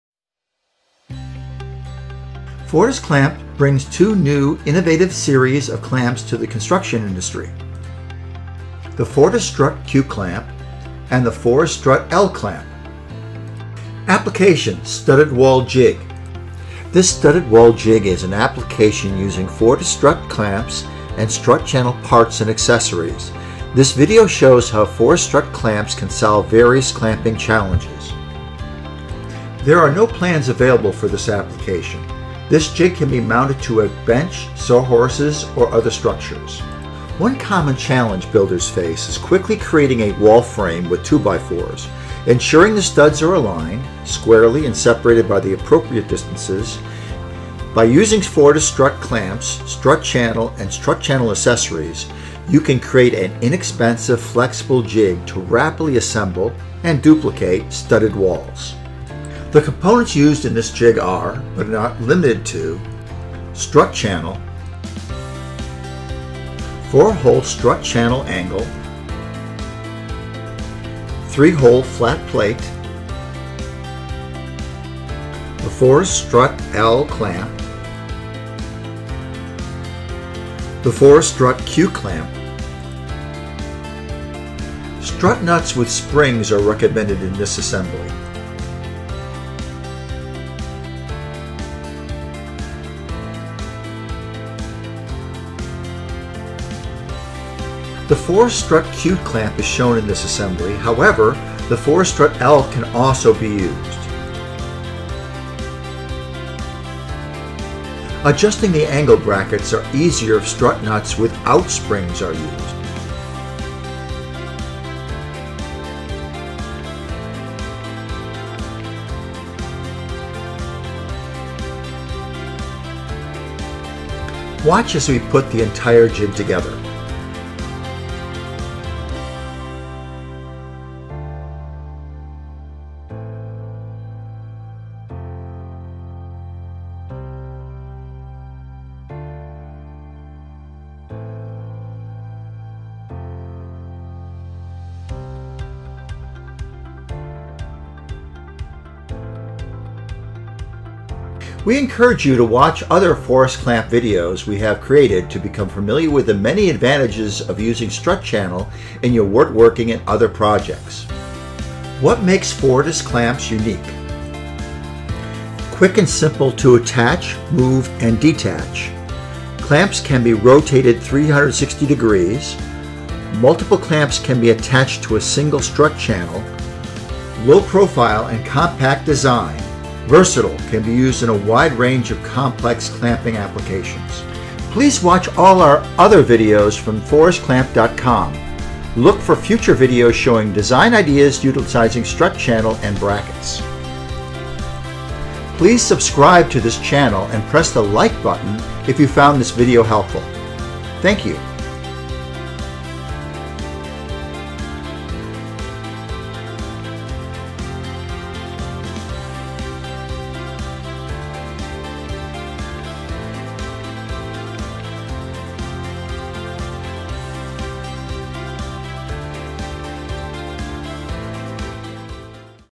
FortisClamp-Studded Wall Jig
This is an application using Fortistrut series of clamps to build a useful Studded Wall jig. Studded walls can be found in shed, garages and homes. It uses strut channel, strut channel brackets, strut nuts and common fasteners found in major home improvement stores. There is no welding. Use a wrench and other common tools to assemble this jig.